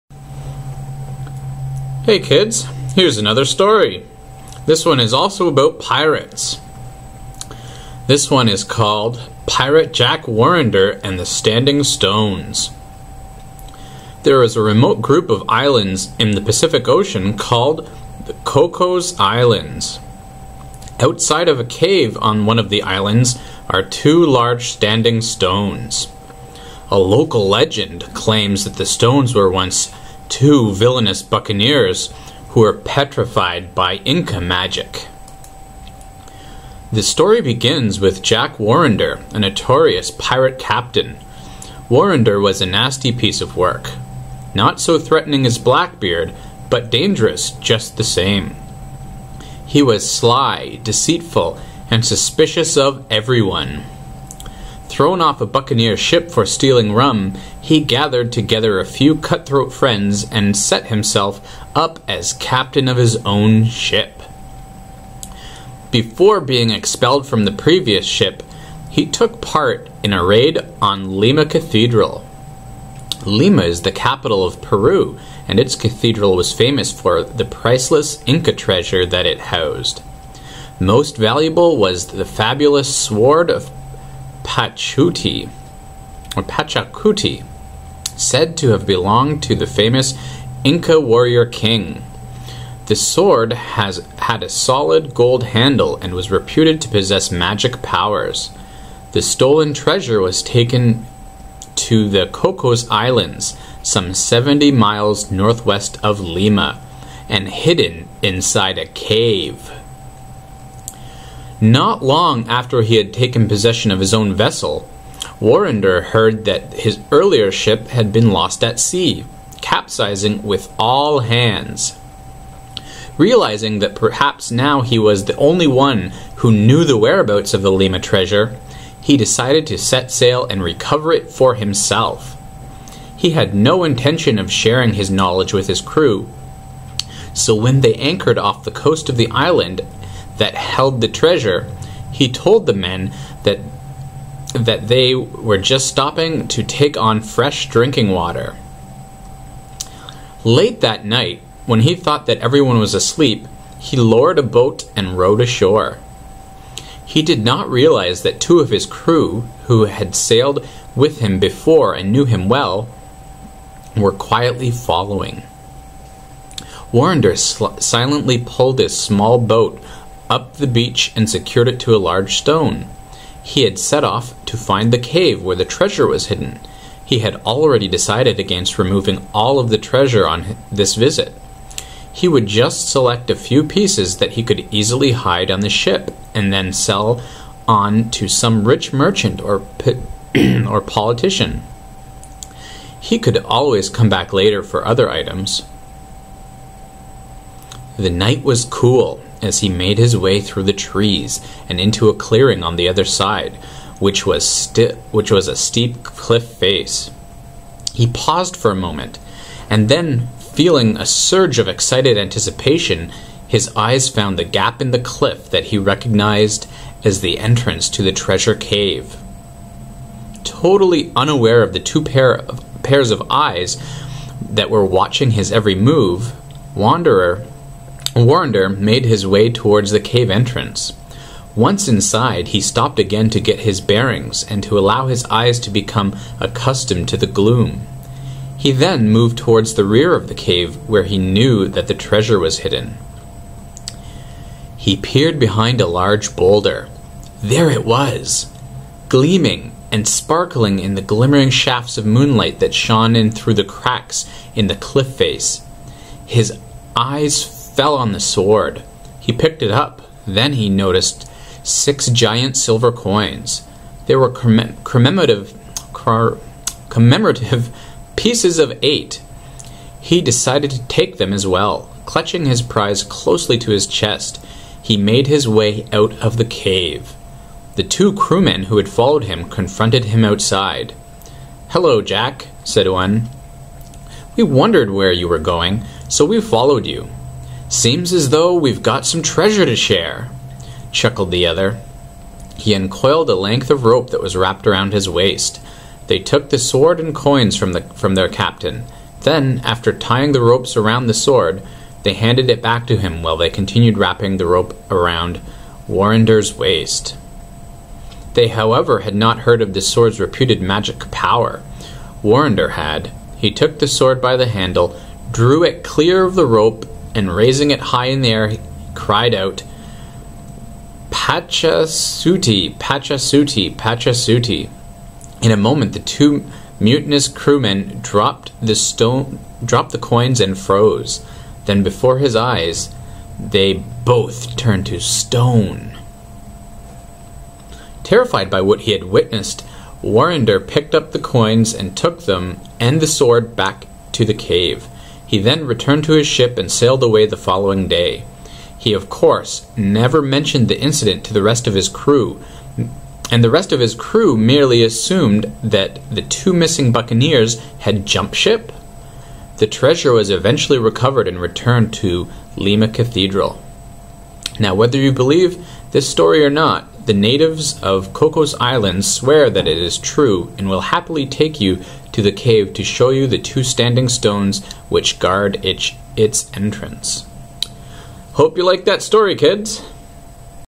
Pirate Jack Warrender and the Standing Stones - Leslie Melville
I read Pirate Jack Warrender and the Standing Stones by Leslie Melville

I hope you enjoy this reading!